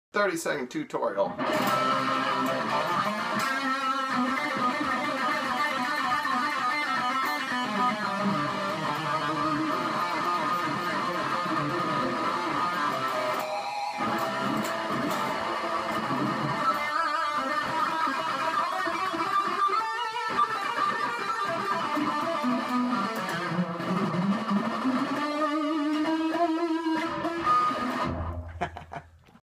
Uncle Fester Shredding
Steve Weinhold from Knights Of The Remnant with a 30 second shred.